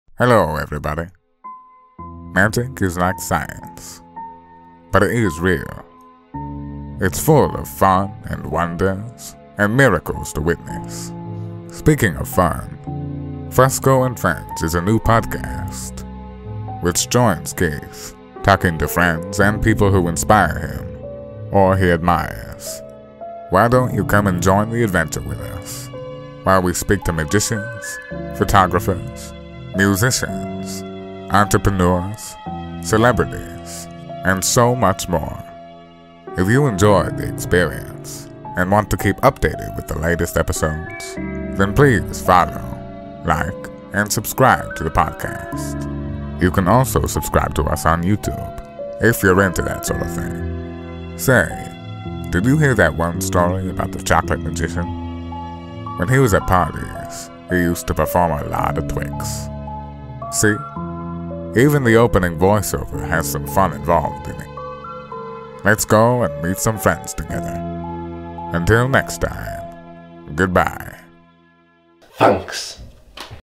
Fusco And Friends | Podcast Coming Soon...
Just a quick introduction from Morgan Freeman, to let everyone know about the Fusco And Friends YouTube channel/podcast which is happening!

Fusco And Friends is a podcast hosted by Keith Fusco and covers a range of subjects and has a host of people from magicians, photographers, musicians, celebrities and much more. Chill out, have some fun and make some new friends with us here at Fusco And Friends.